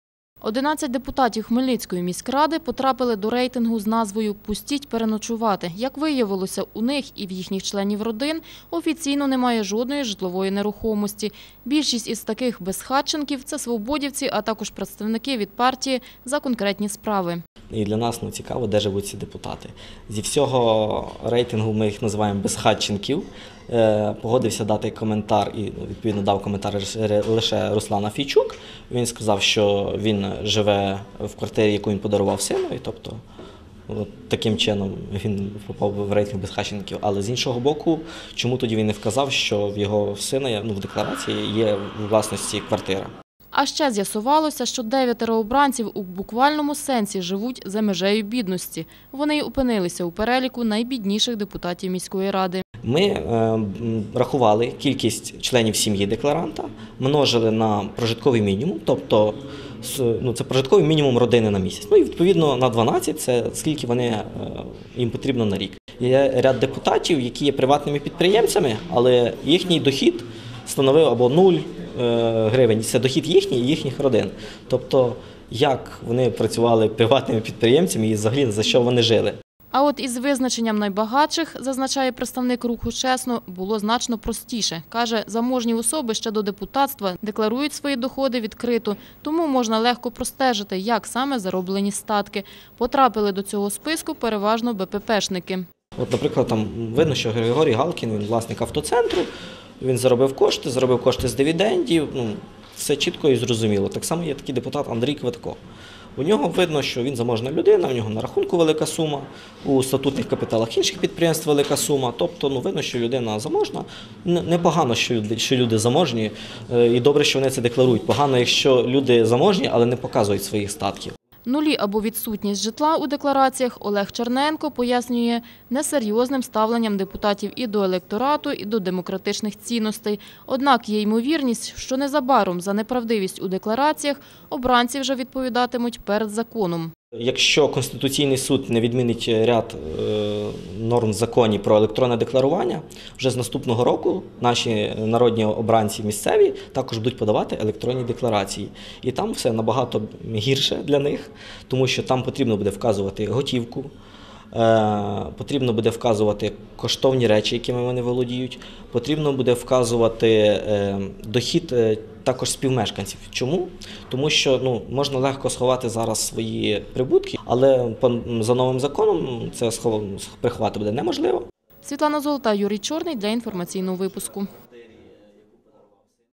Безхатьки, багатії і бідняки депутати Хмельницької міської ради
Серед депутатів Хмельницької міської ради є чимало безхатченків. Принаймні, такого висновку дійшли активісти громадського руху «Чесно». У рамках своєї кампанії «Стеж за грошима», вони, аналізуючи декларації обранців про доходи та майно за минулий рік, склали відповідні рейтинги. До прикладу, про найбідніших і найбагатших.